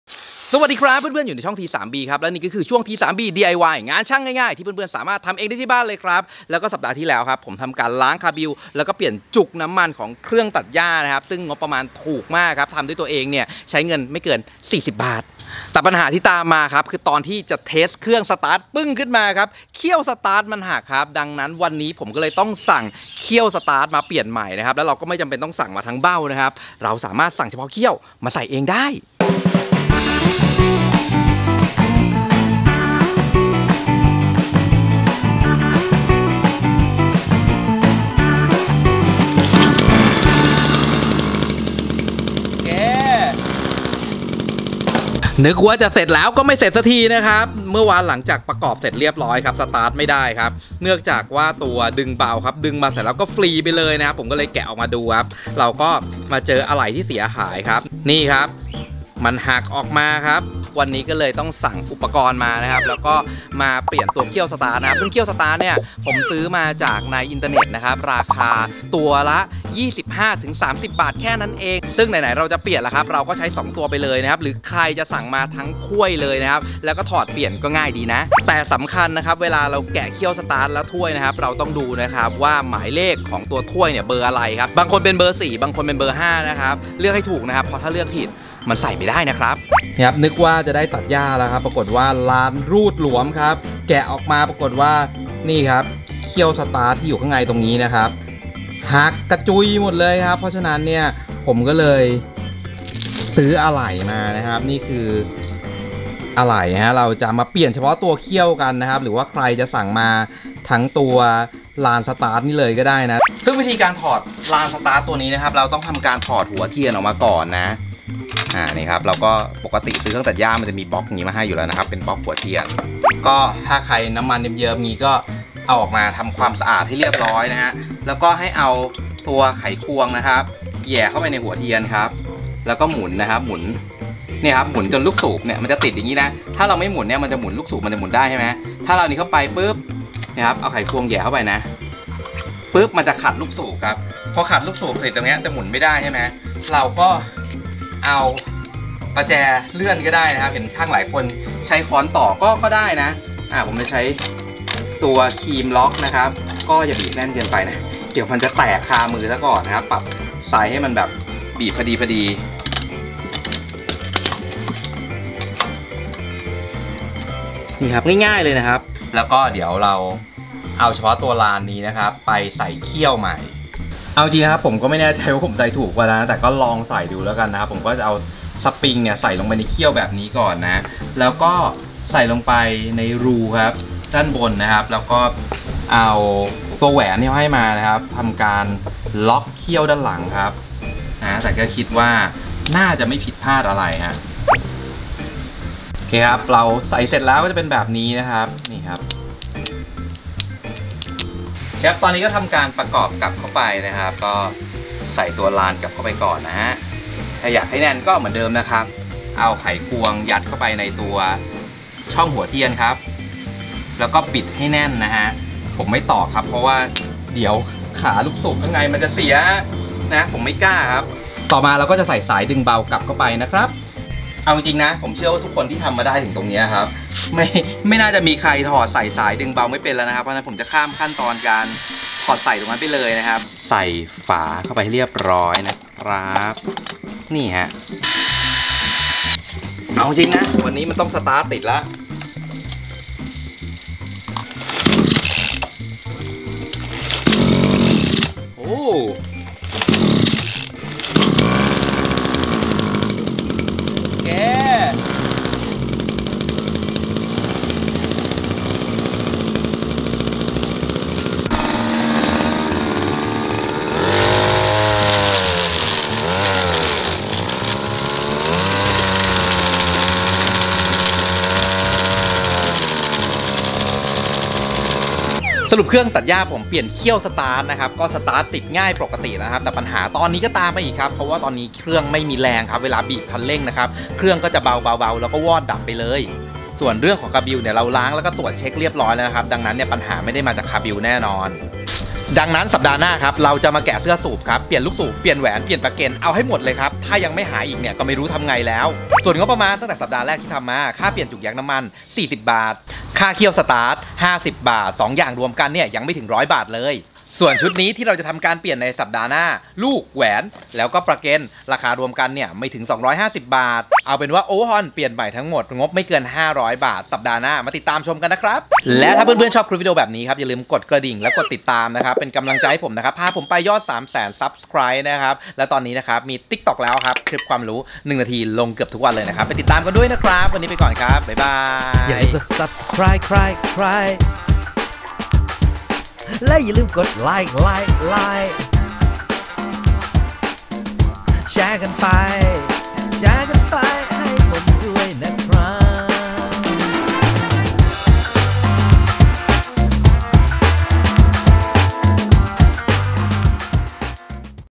ซ่อมเครื่องตัดหญ้า EP2 เปลี่ยนเขี้ยวลานสตาร์ท l T3B DIY
ใช้งานมานาน 1 ปีก็ต้องมีเสียบ้าง อาทิตย์ก่อนเปลี่ยนยางแย๊กล้างคาบู แต่เขี้ยวสตาร์ทดันหักซะได้ เปลี่ยนเองอีกเช่นเคย ง่ายนมากๆ  ถูกว่าไปร้านซ่อมแน่นอน อะไหล่ใช้กับ CG260 ได้ถูกมากๆ
shopee
ลูกสูบ+แหวน ราคา 80 บาท
เบ้า+เขี้ยวสตาร์ท no4 เครื่อง 260 ราคา 35 บาท
เครื่องตัดหญ้า Mitsumax ราคา 1,700
จุกแย๊กน้ำมันราคา 4 บาท

Lazada
เครื่องตัดหญ้า 2 จังหวะ CG260 ราคา 1,790
จุกแย๊กน้ำมันราคา 10 บาท